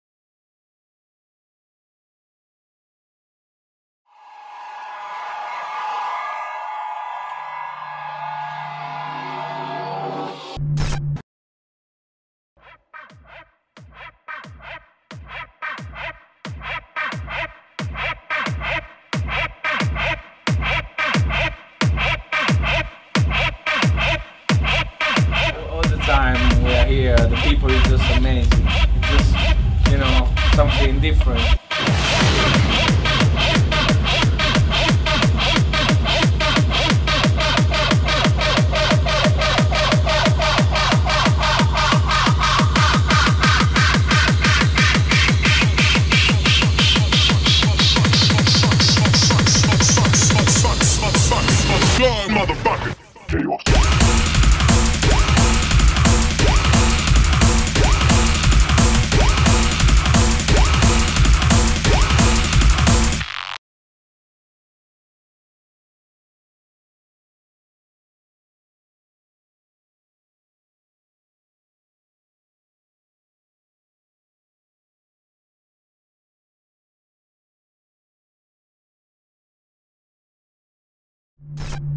Video flyer Hallucinator @ Melting Pot 8th Anniversary @ LOUD (Guadalajara, SP)
LOUD regresa para ofreceros un menú cargado de ritmos rotos con HALLUCINATOR como plato fuerte del banquete de aniversario de MELTING POT RECORDS.

Este dúo italiano, que lleva años paseando su maldad por medio mundo, hace escala en Guadalajara para hacer saltar los empastes de los asistentes, todo un honor compartir esta velada con unos de los máximos exponentes actuales de la vertiente más macarra del drum&bass.